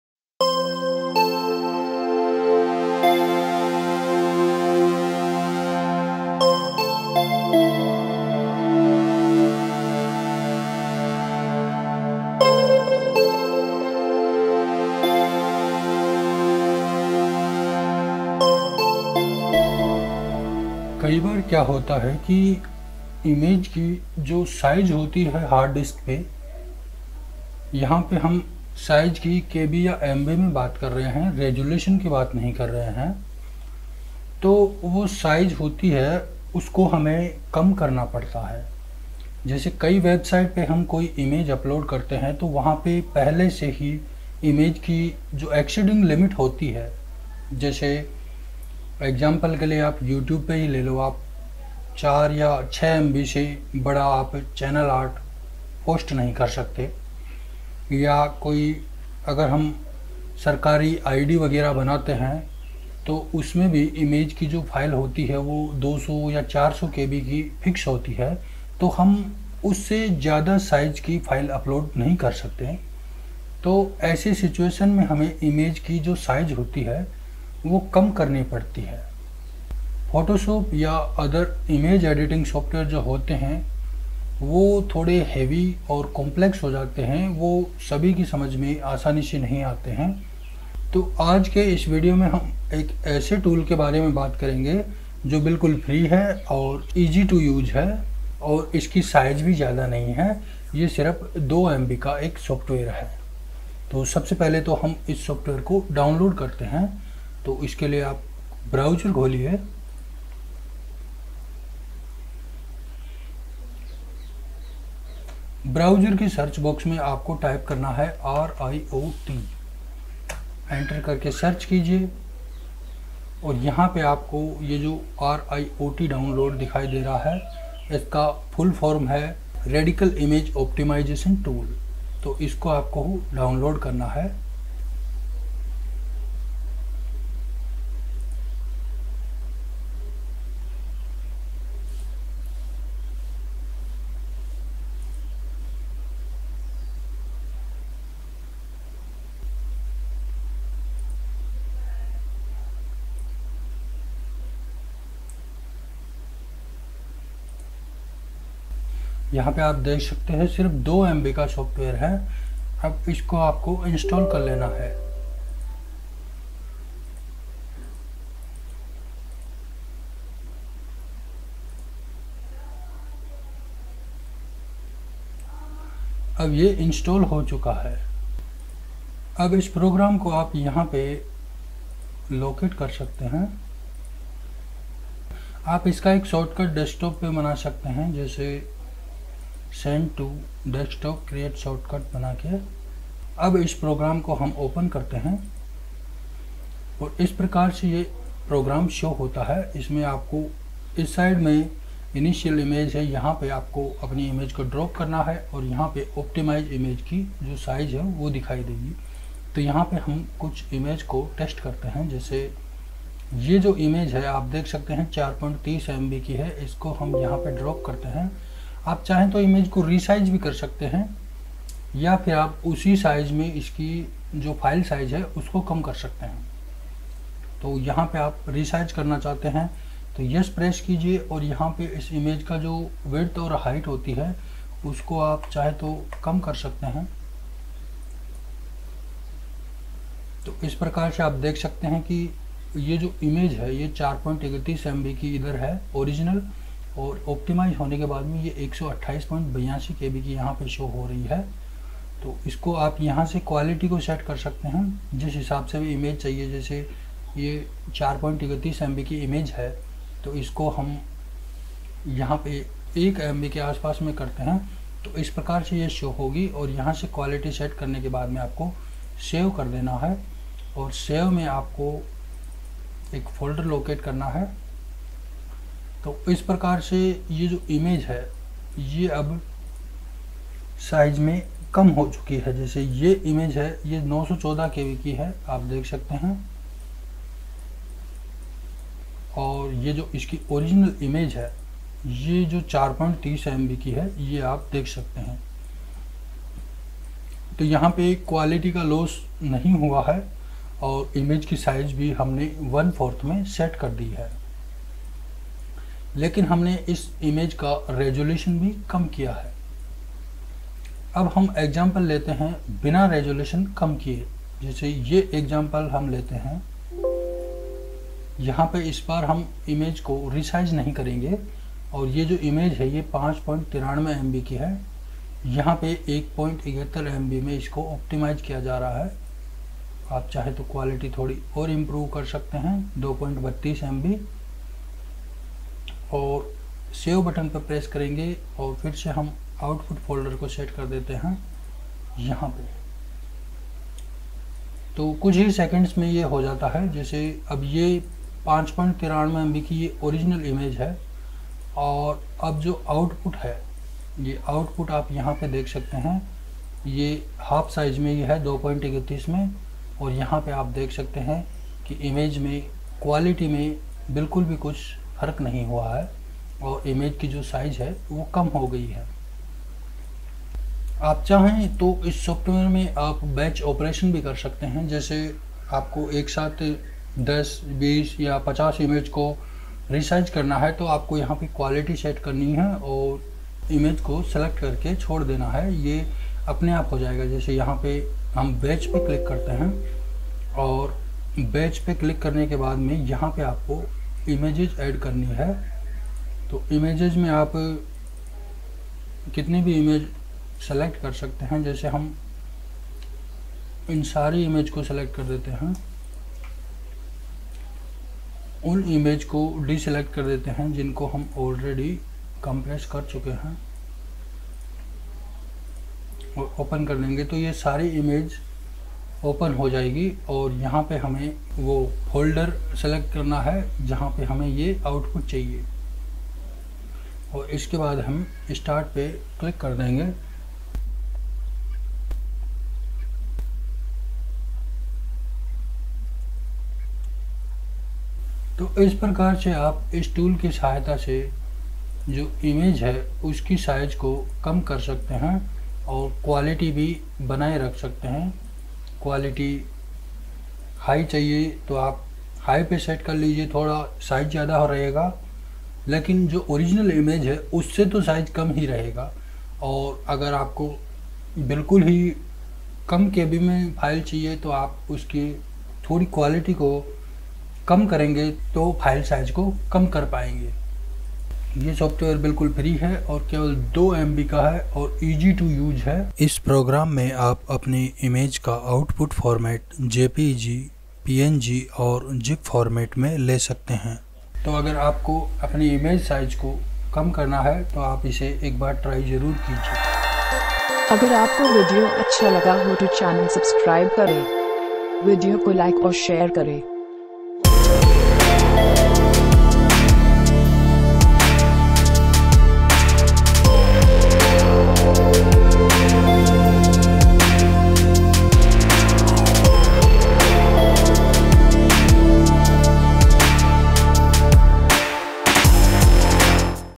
Compress or Resize Images Without Losing Quality Offline
In this video I will show you how to compress image size in just few seconds by using RIOT.
RIOT is free and easy to use image resize and image compression tool.
by using this you can compress image size 40 to 80 percent without loosing quality.
इमेज को compress कैसे करें। How to compress image size .
How to Compress Image Size Without Losing Quality Offline in Hindi